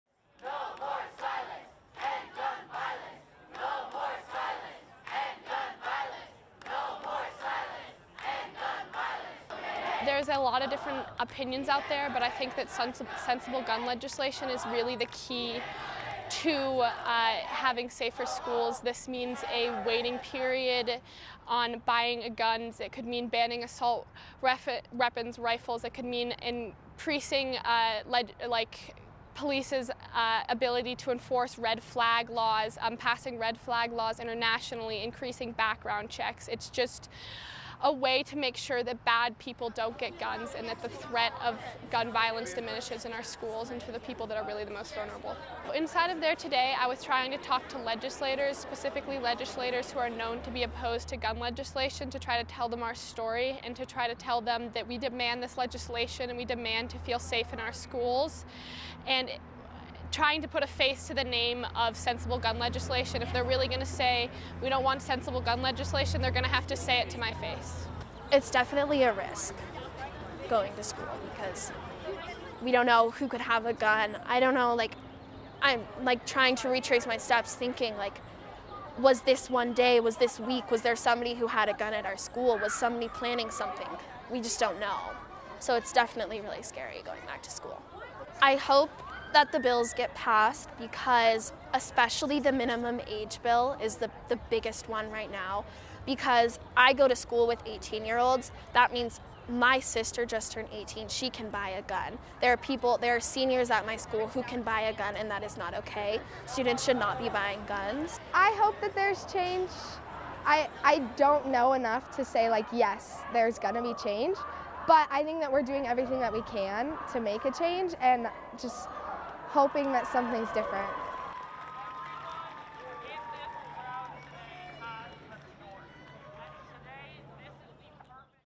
Students, parents protest Denver school shooting
(24 Mar 2023) Students and parents fed up with gun violence after a shooting injured two administrators at a Denver high school gathered to protest at the Colorado State Capitol building Thursday. (March 23) (AP video/Thomas Peipert)